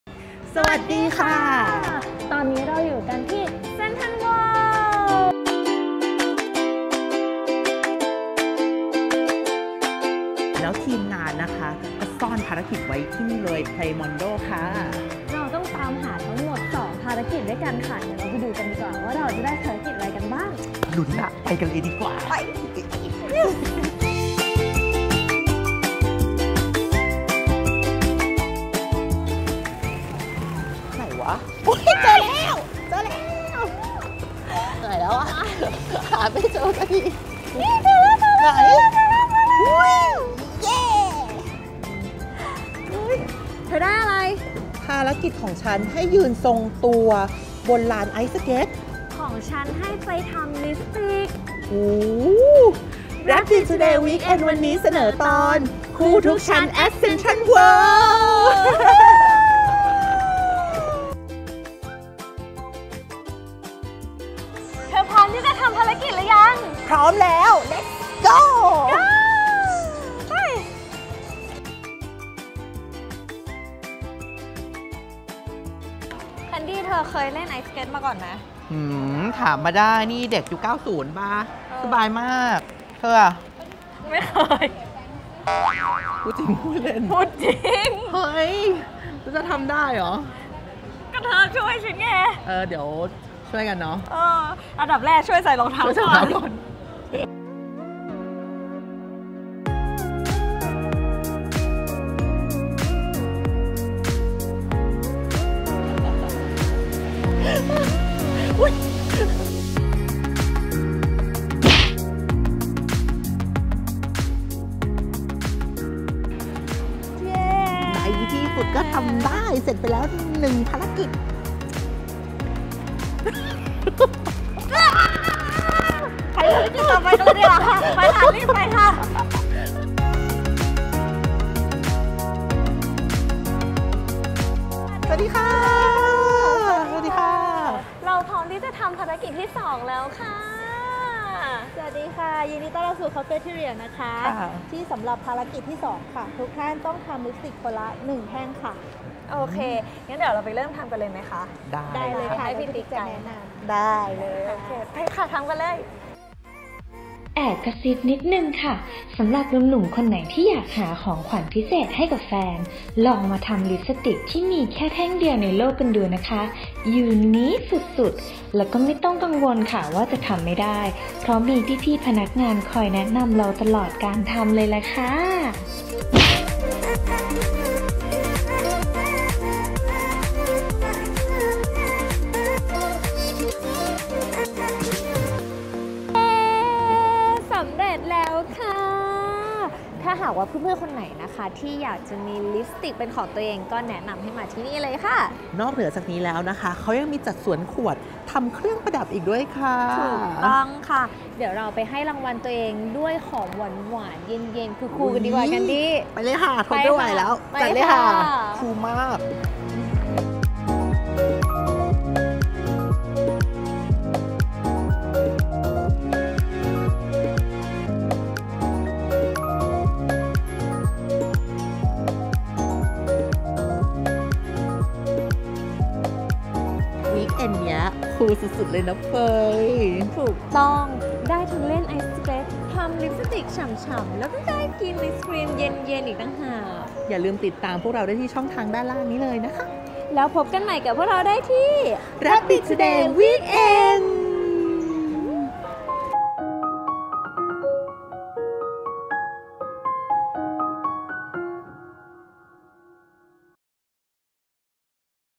NJ TRAVEL Episode 4: ลุย CTW ห้างใหญ่ใจกลางเมือง
Weekend นี้ NJ Digital และสองสาว ‘เฟย’ กับ ‘แคนดี้’จะพาไปเที่ยวห้างกัน แต่แค่ไปห้างเฉยๆ มันก็คงจะธรรมดาไป๊ เพราะฉะนั้นแน่นอนว่าพวกเธอทั้งสองจะมาพร้อมกับภารกิจป่วนๆอีกเช่นเคย แต่จะเป็นอะไนบ้างนั้น ต้องดูกันเองเลย